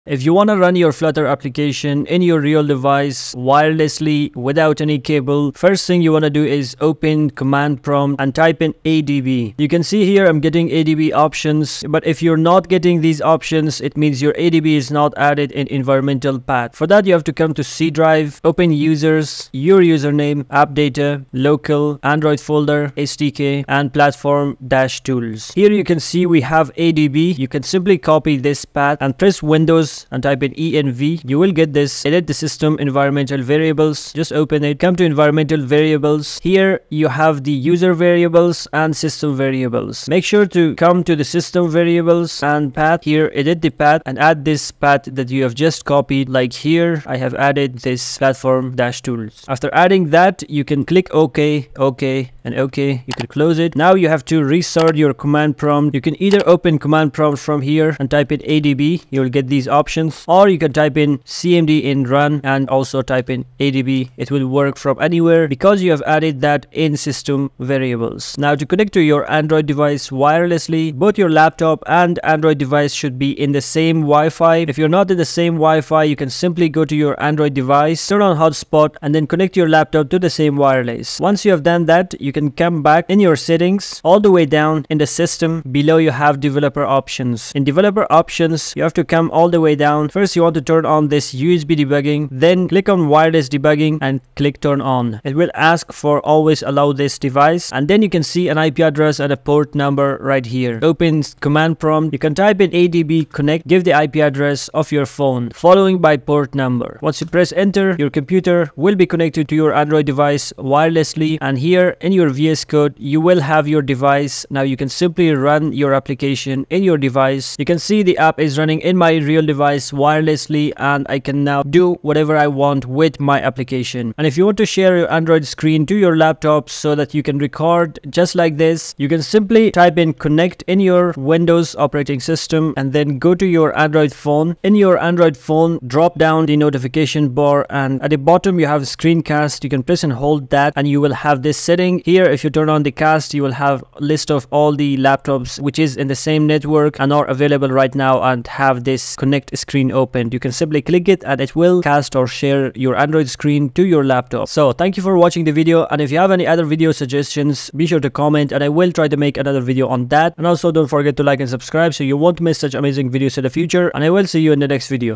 Run Flutter Apps on Real Devices Wirelessly & Screen Cast Android to Laptop
In this video, I’ll show you how to run your Flutter application wirelessly on a real Android device—no cables needed! 🚀 You’ll also learn how to screen cast your Android phone to your laptop for easy recording or presentation.

Here’s what we cover:
🔹 Setting up ADB in your system’s environment variables 🛠️
🔹 Connecting your Android device wirelessly to your laptop via Wi-Fi or hotspot 🌐
🔹 Running your Flutter app directly from VS Code on your real device ⚡
🔹 Sharing your Android screen to your laptop for screen recording or demo presentations 🎥

Be sure to watch till the end to learn all the steps! If you enjoyed the video or have any questions, leave them in the comments below.